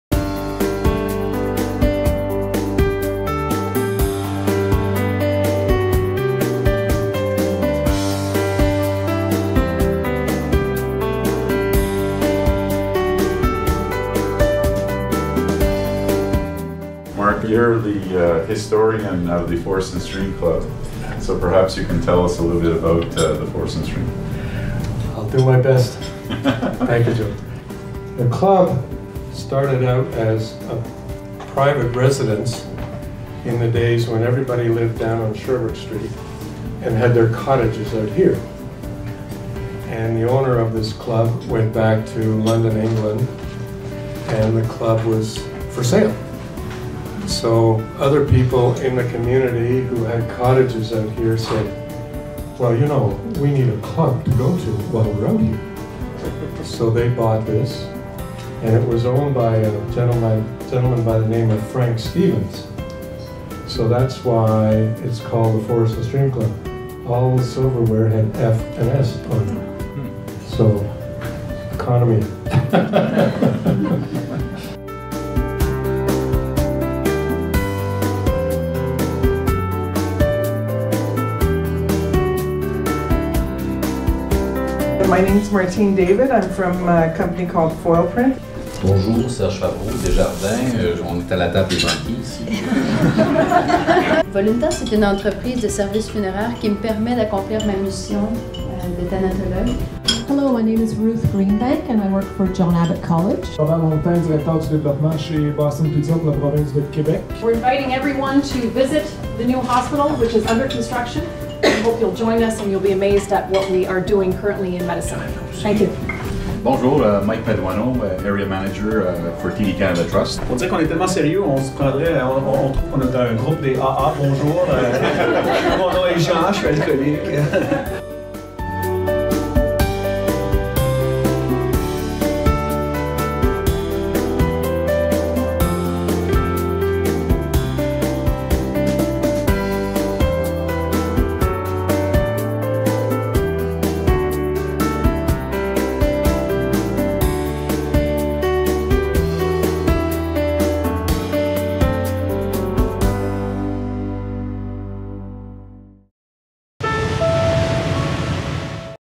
Les rendez-vous TD Canada Trust Déjeuner Réseautage / Networking Lunch
Fantastic networking lunch at the Forest & Stream Club in Dorval.